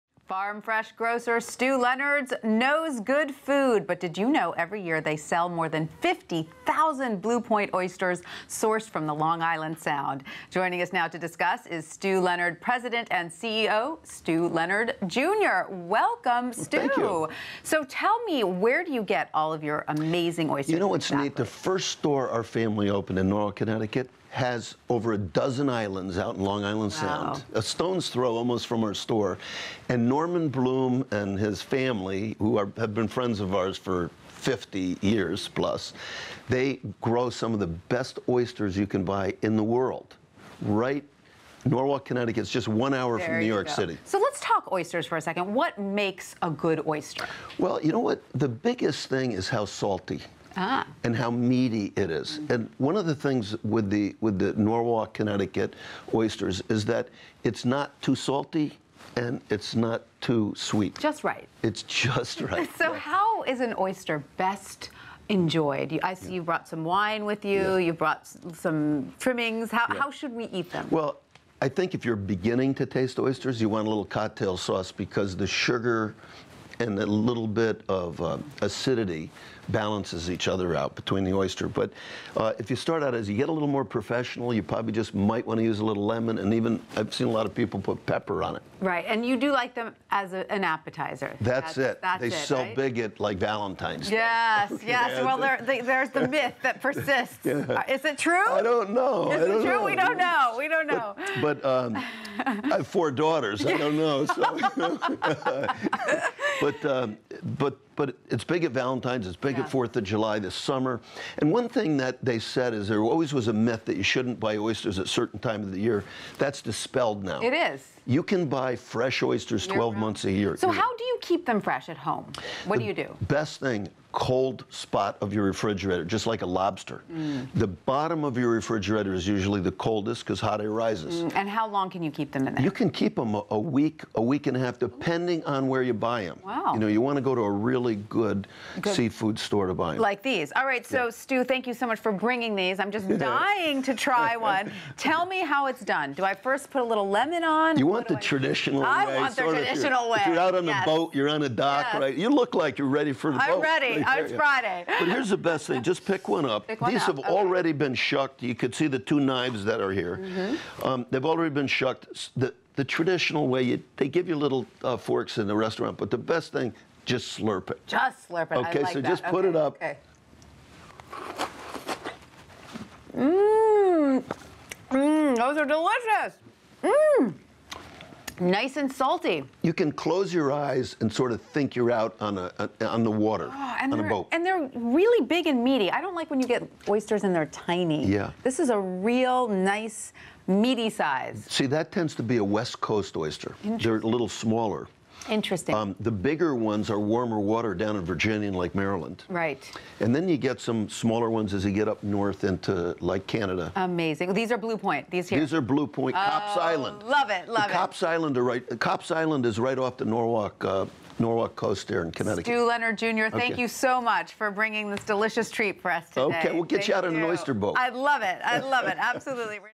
How to Enjoy Blue Point Oysters
Stew Leonard’s CEO Stew Leonard Jr. joins Lunch Break with tips on how purchase, cook and pair blue point oysters for a delicious meal. Photo: Stew Leonard's